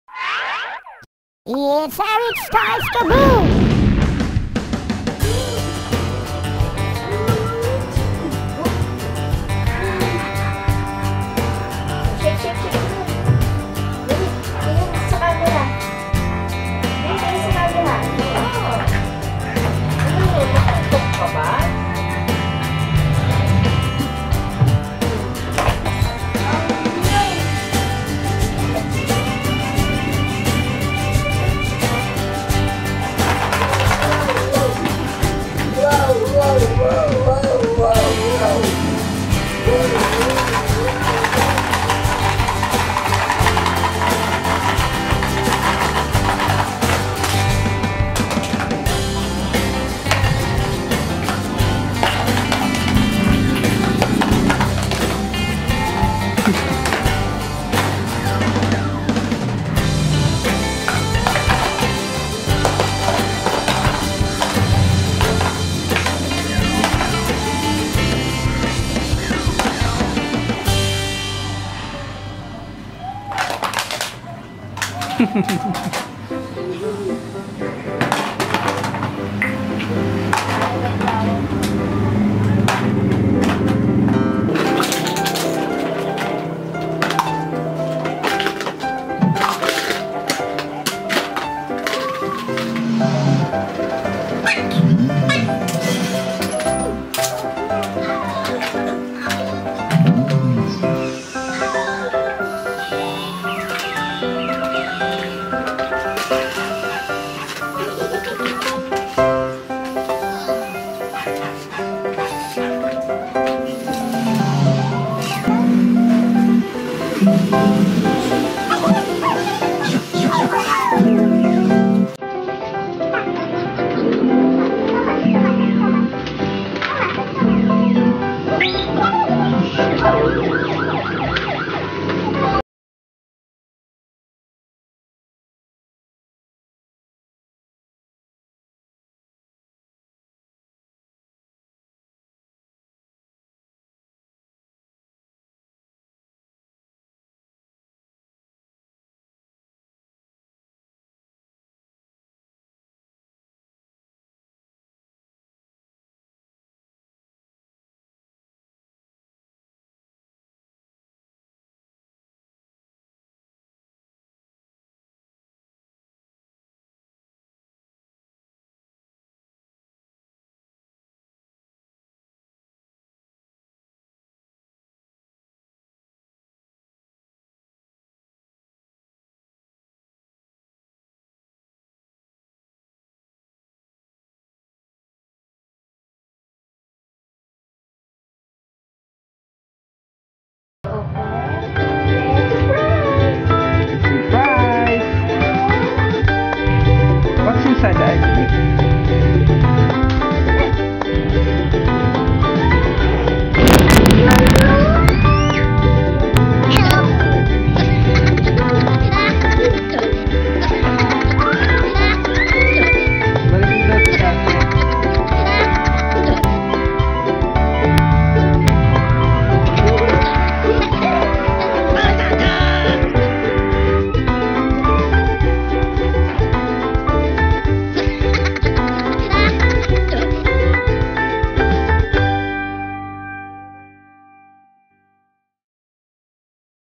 Lots of Baby toys, Robocar Poli Movie, Playthings for Kids, Playtime with Thomas Tank Engine.
After Robocar Poli movie, It's another Playtime with kids and it's time to pour out the playthings or simply lots of baby toys. This is a toy adventure of Aric Boy with Thomas the Tank Engine, etc. We can also see RC Helicopter Toy, micro cars, trucks and many more toys for toddlers.